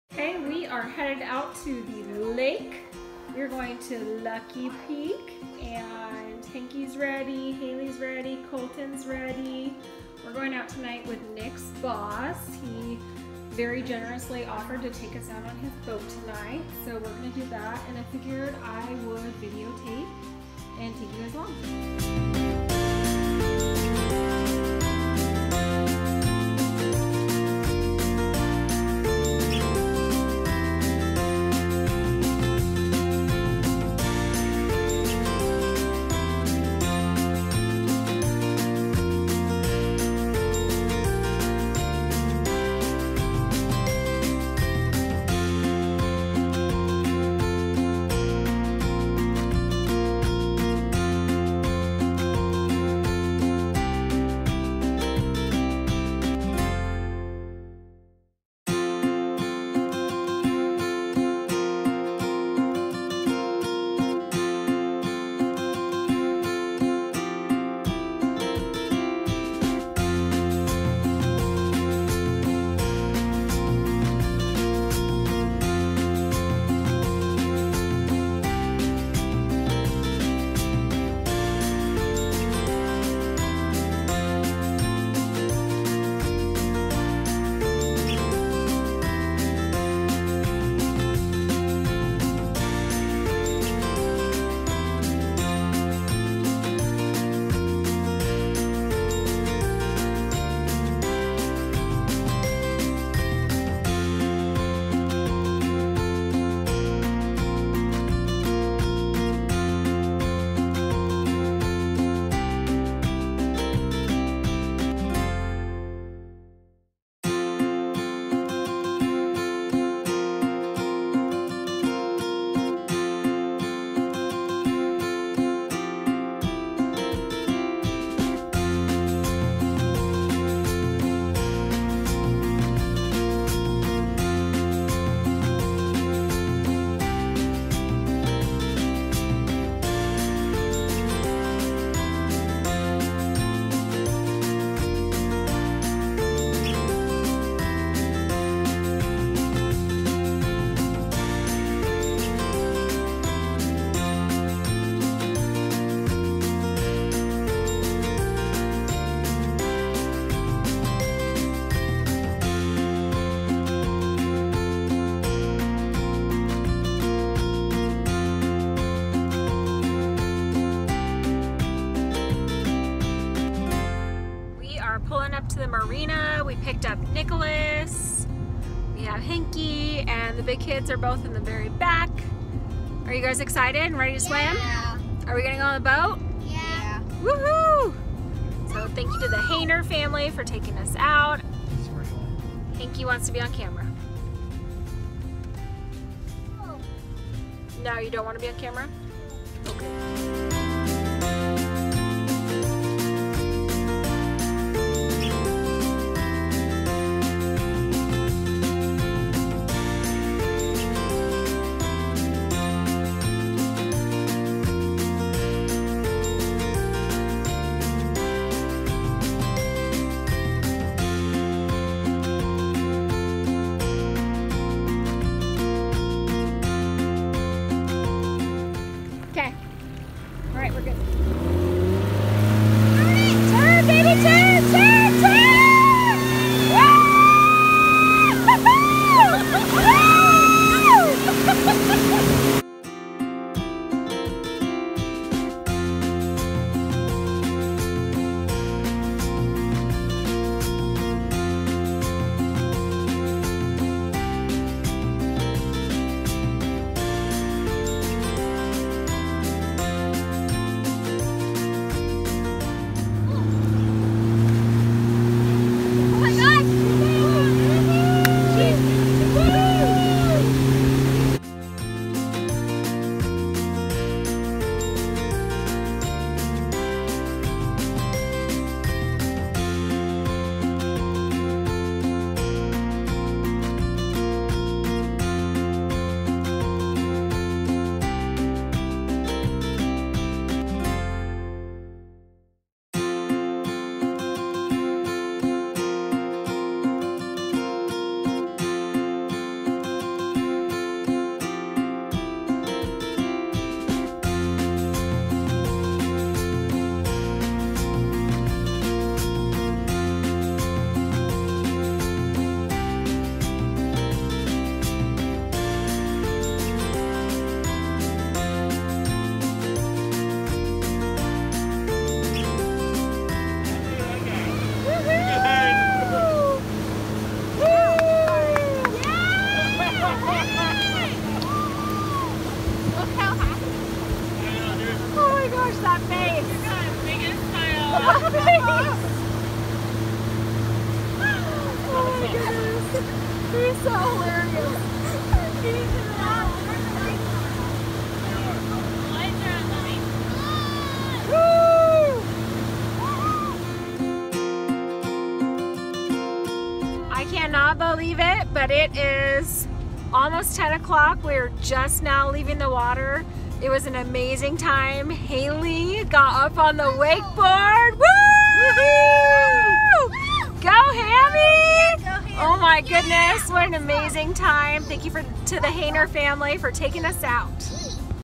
LUCKY PEAK l LAKE LIFE l HAILEY WAKEBOARDS!!
Check out beautiful Lucky Peak Reservoir with us! Hailey wakeboards like a champ and Hank goes tubing with big bro for the first time! Great night with friends!